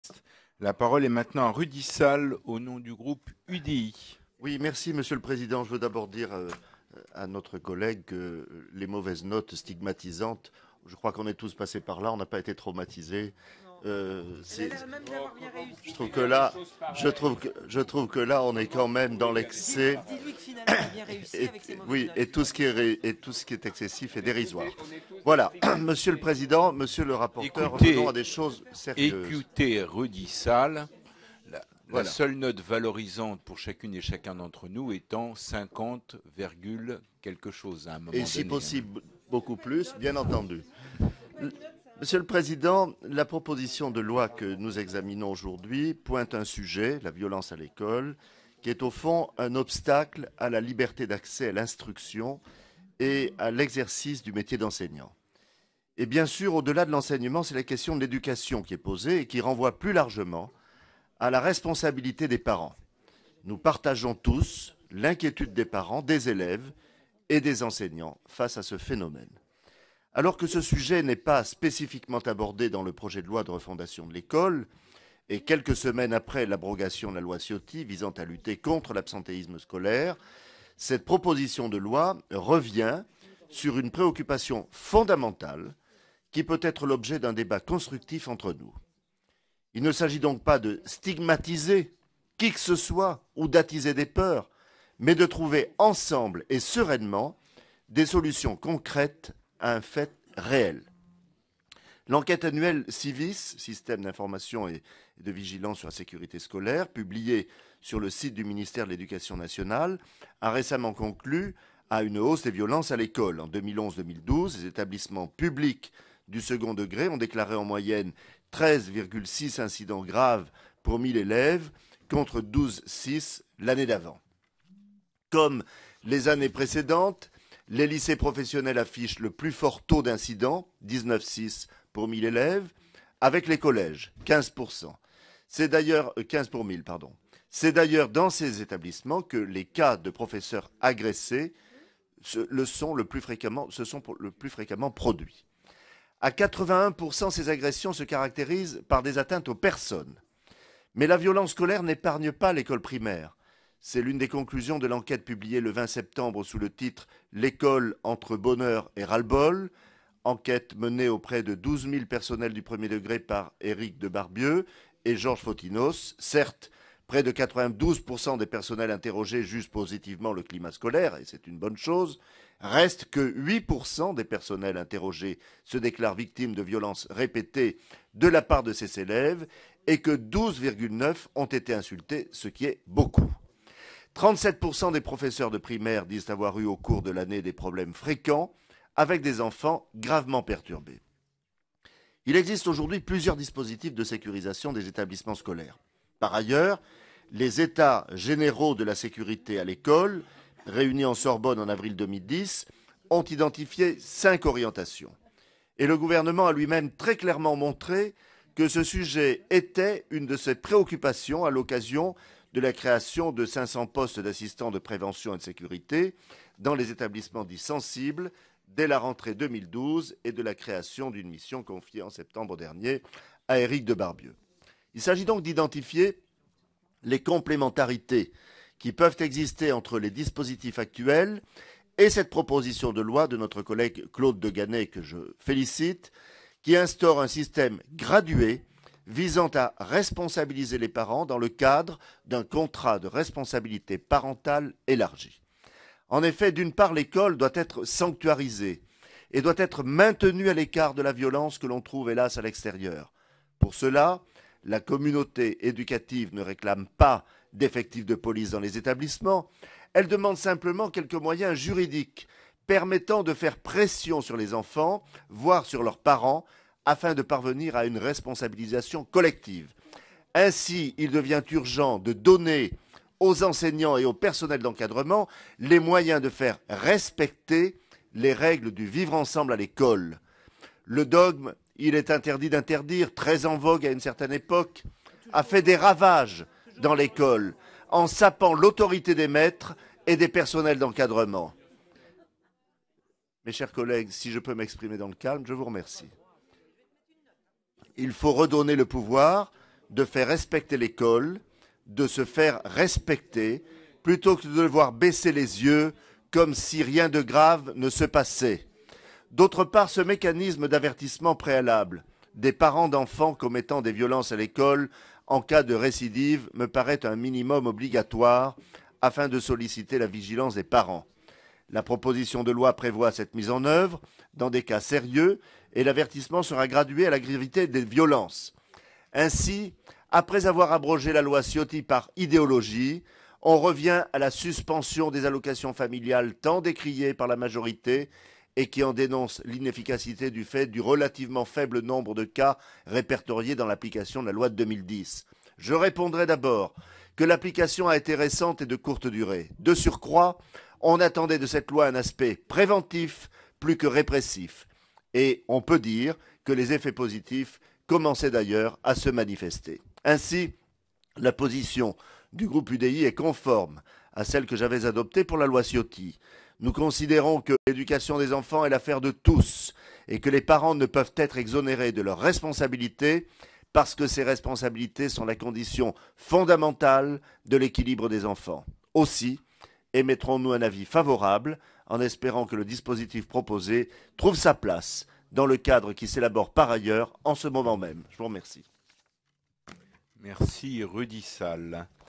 2013 02 13 AN Rudy Salles soutient la proposition de loi contre la violence en milieu scolaire
Ce matin en Commission des Affaires culturelles et de l'Éducation, Rudy Salles est intervenu au nom du groupe UDI pour soutenir la proposition de loi du député Claude de Ganay visant à prévenir et lutter contre la violence en milieu scolaire.